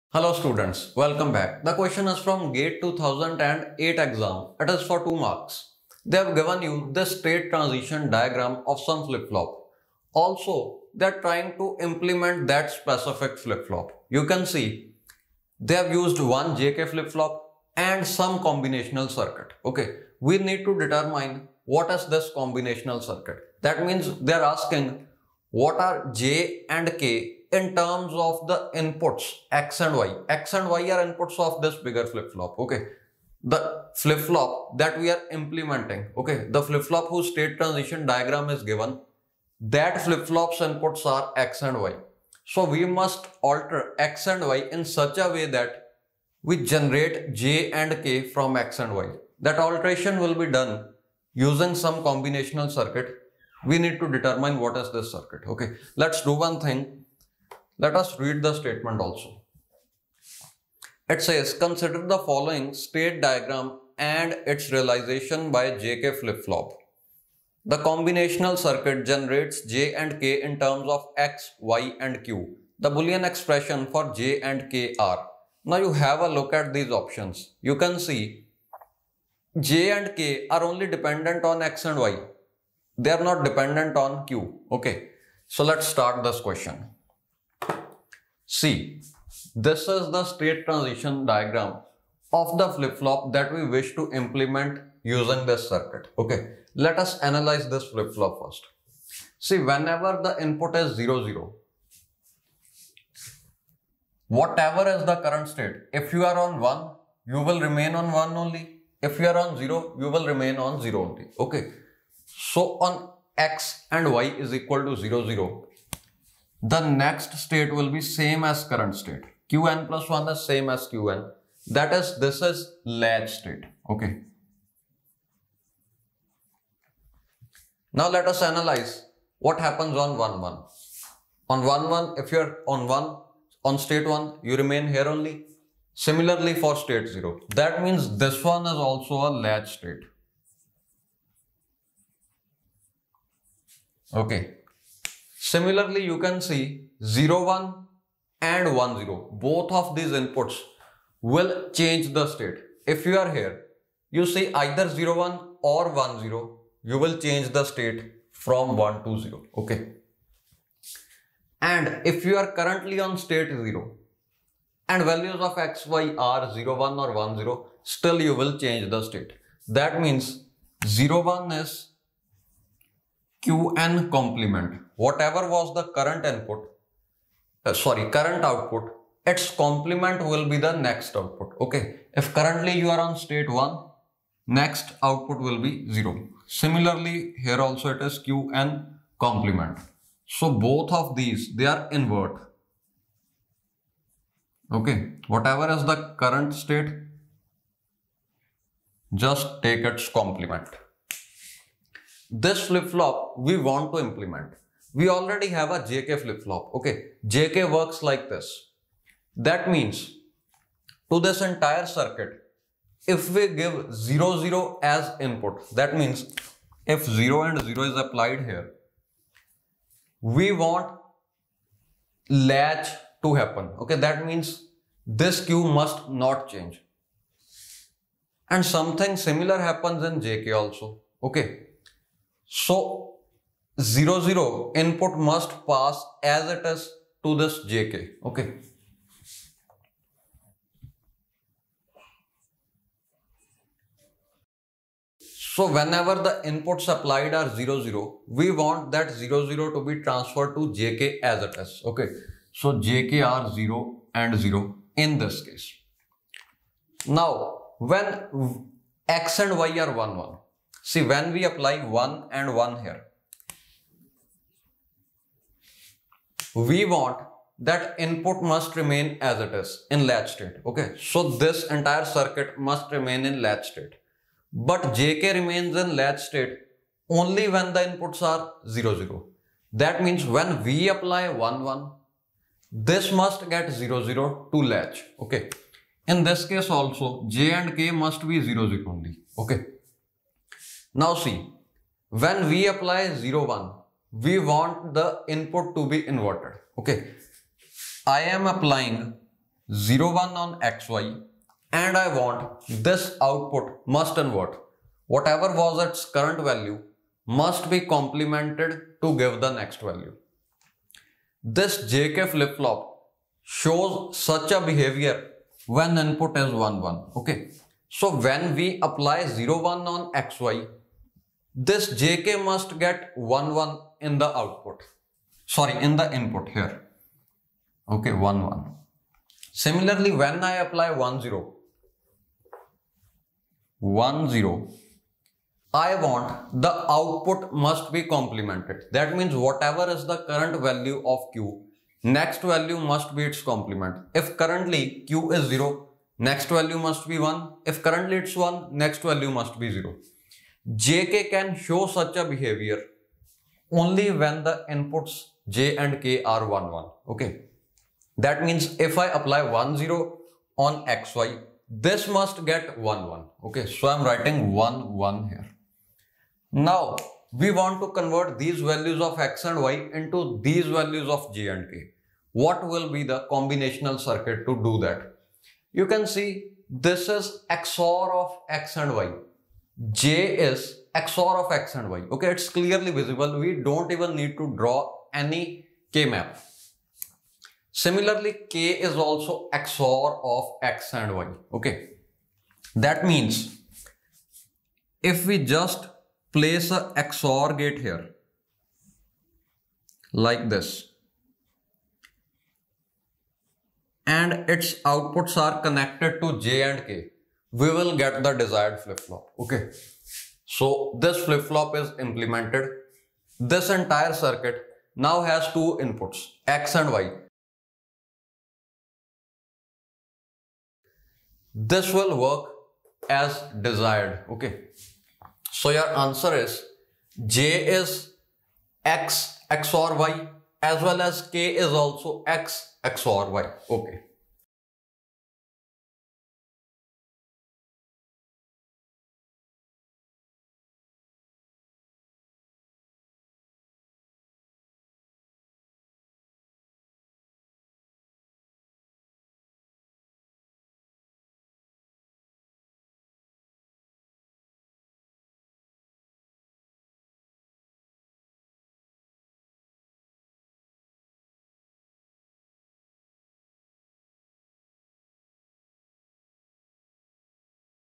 Gate 2008 pyq DIGITAL | Consider the following state diagram and its realization by a JK flip flop
Consider the following state diagram and its realization by a JK flip flop

IMAGES NOT SUPPORTED

The combinational circuit generates J and K in terms of x, y and Q.
The Boolean expressions for J and K are :
(A) (x⊕y)’and x’⊕y’
(B) (x⊕y)’and x⊕y
(D) x⊕yand x⊕y